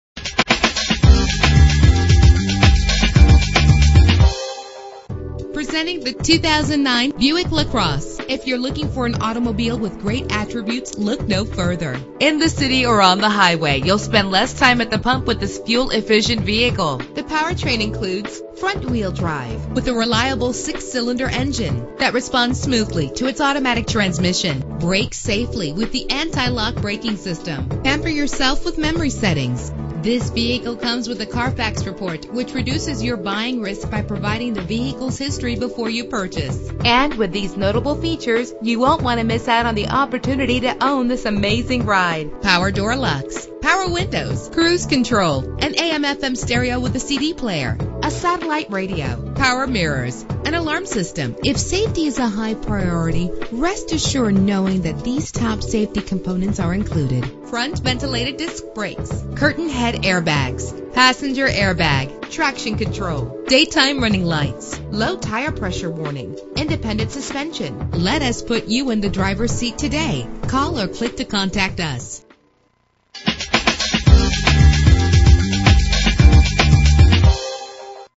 2009 Buick LaCrosse - Avenel New Jersey
Year: 2009
Make: Buick
Model: LaCrosse
Trim: CX
Engine: 3.8 liter 6 cylinder 12 valve
Transmission: 4-Speed Automatic
Color: Beige
Mileage: 50698
Address:
90-100 Route 1 North